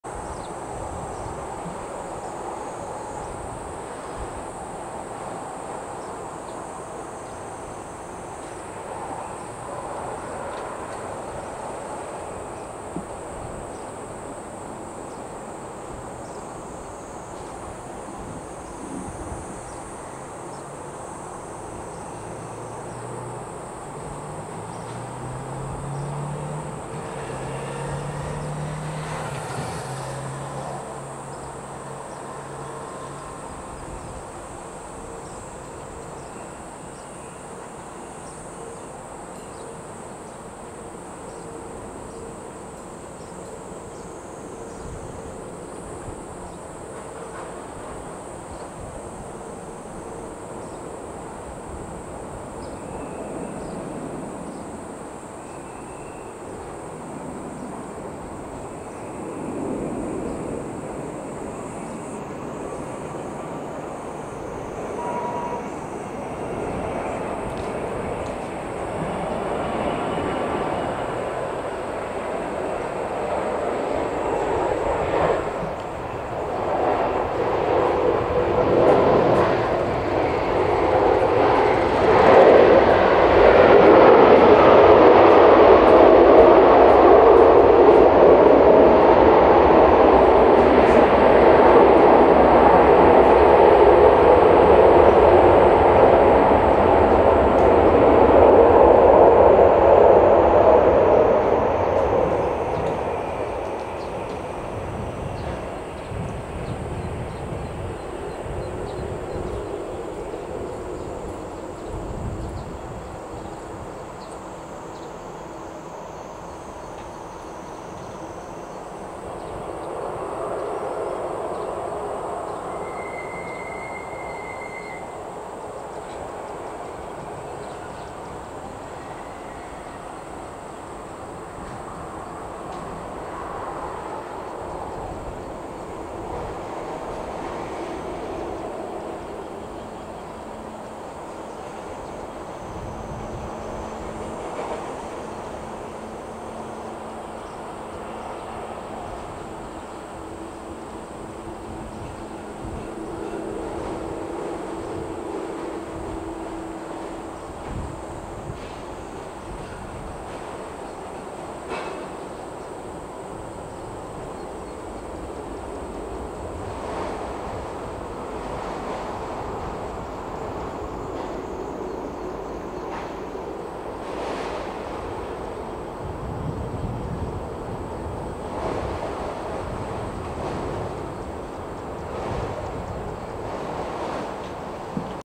【R2-D2】ANA B789 (JA873A - StarWars R2-D2 Livery) NH204 FRA-HND RWY22 Landing
ANA B789 (JA873A - StarWars R2-D2 Livery)
NH204 FRA-HND
RWY22 Landing

撮影日；2022/7/31